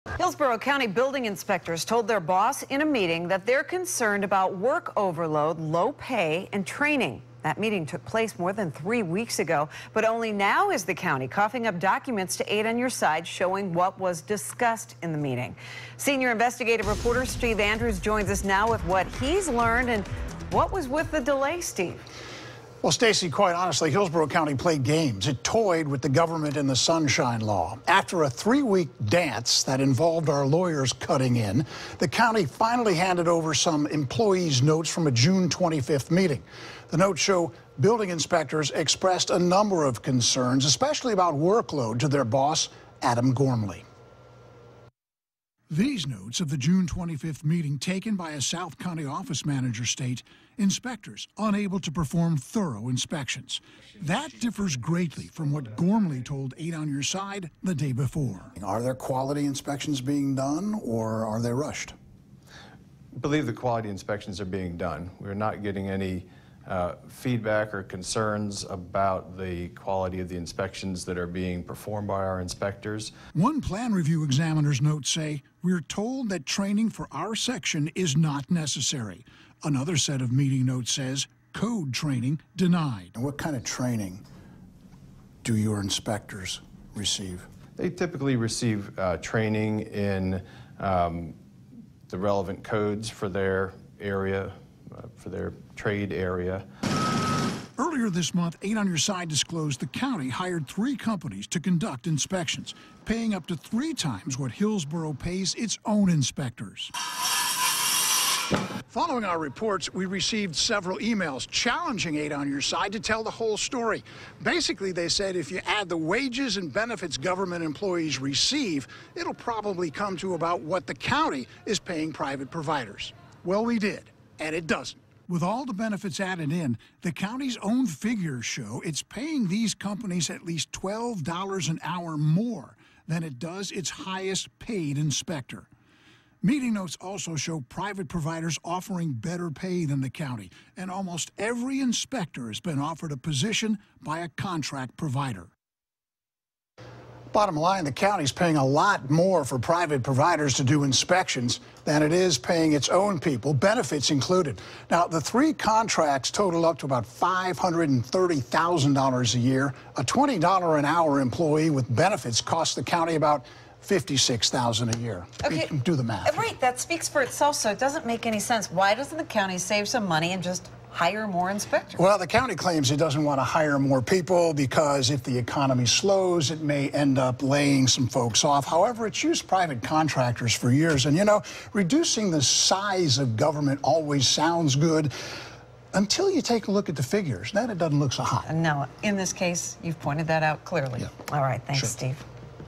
Building inspectors concerned about workloads
Hillsborough county building inspectors told their boss, in a meeting, that they are overloaded with work, their pay is low and they are concerned about training.